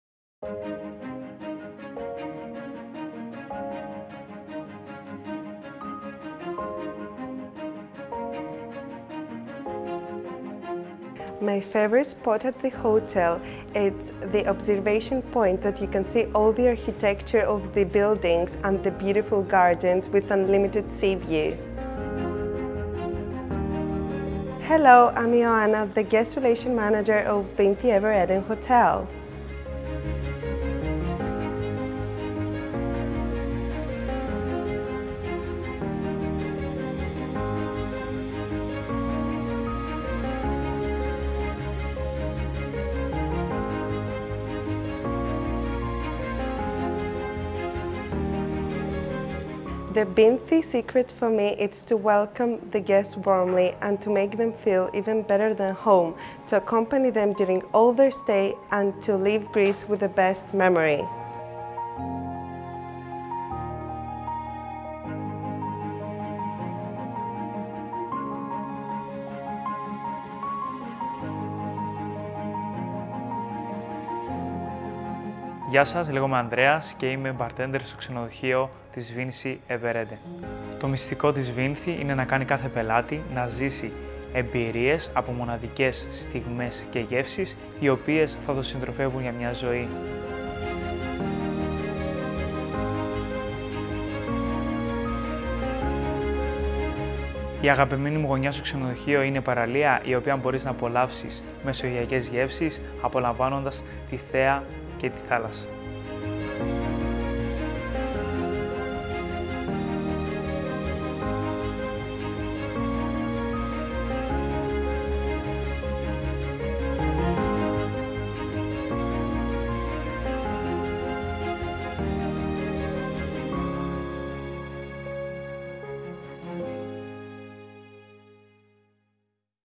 Vincci EverEden Atenas 4* (ENG) | Vincci Hoteles
Our hotel is located in the heart of the Athenian Riviera, 1 hour from the centre of the Greek capital and 35 minutes from Athens International Airport. But it is not only fabulous for its location, behind its walls you will find rooms with all luxury details, exclusive services and a delicious gastronomy that will provide you with a perfect tourist experience.

It is an exclusive resort set in the heart of nature, with direct access to the paradisiacal EverEden Beach, where you will find our Vincci EverEden Beach. Here you can not only disconnect and relax, but also enjoy the best water sports.